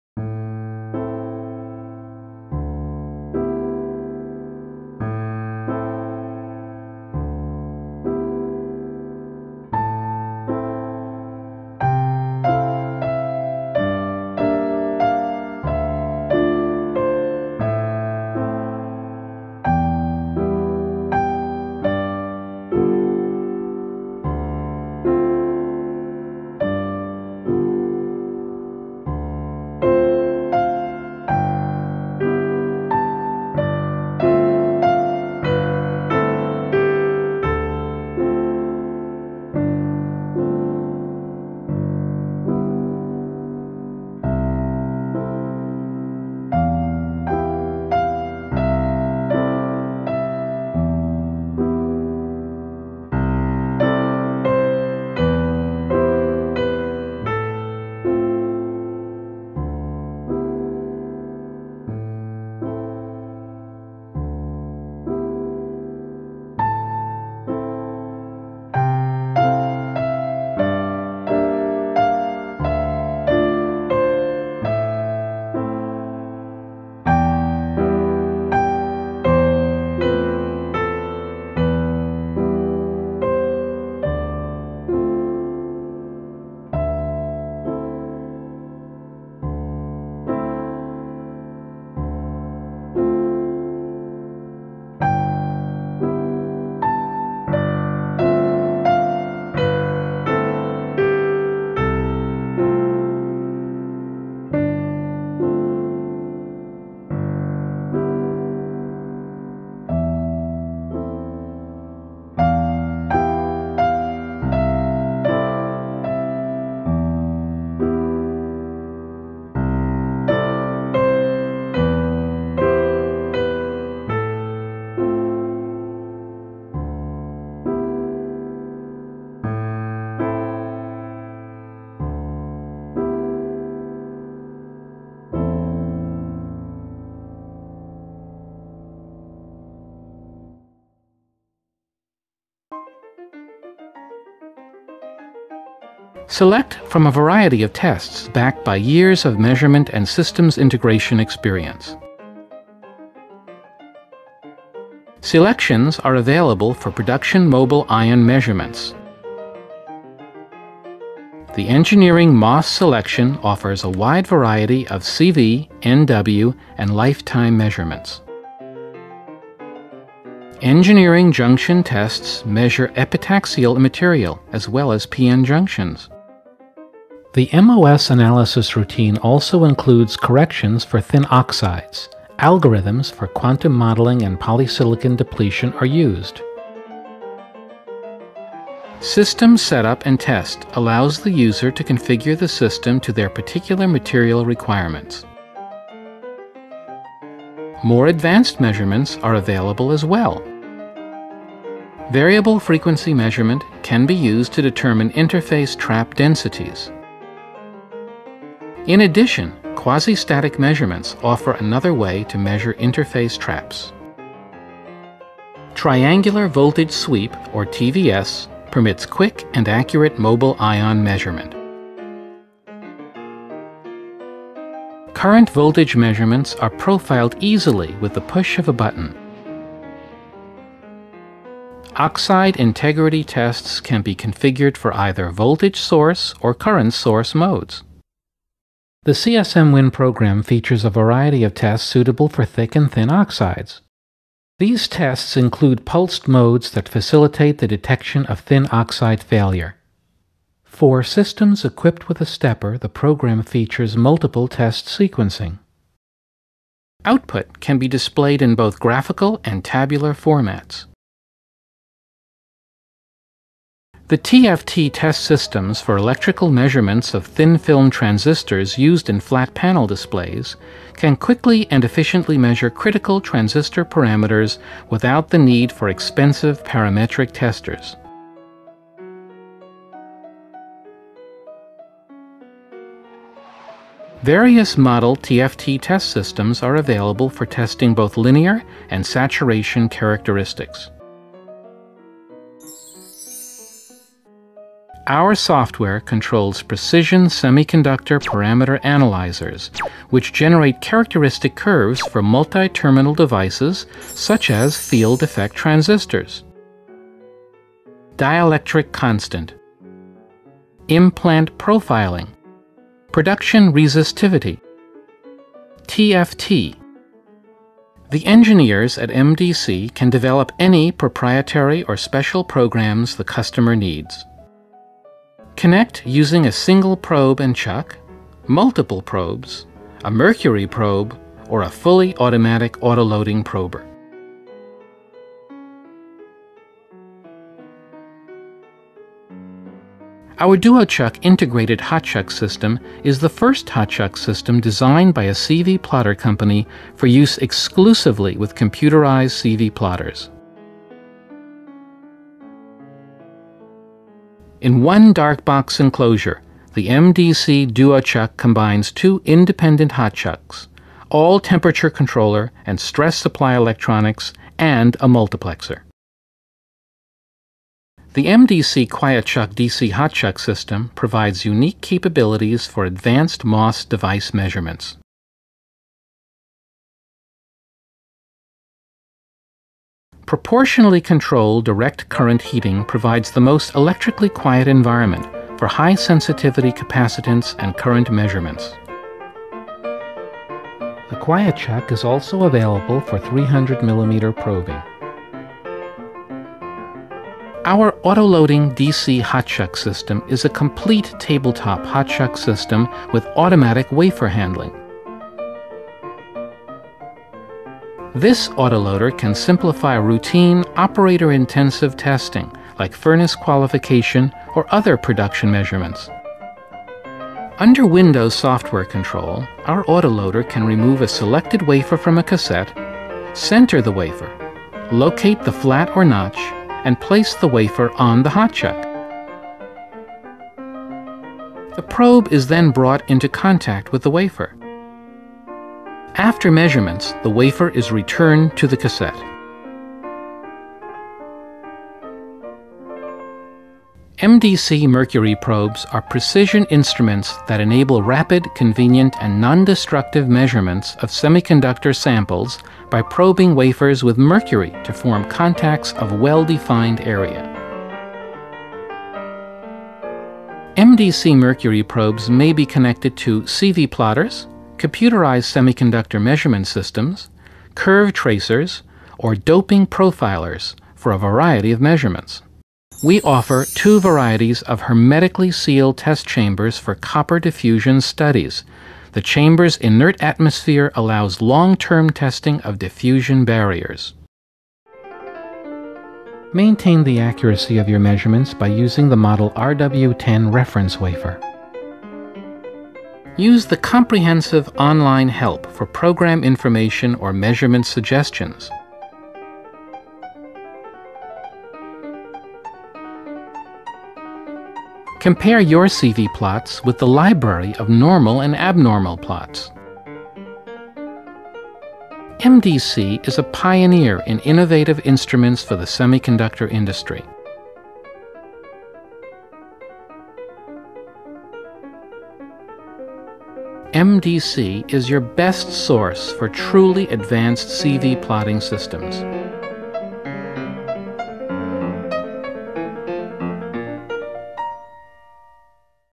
MDC Movie
Features of MDC software and hardware for semiconductor measurements.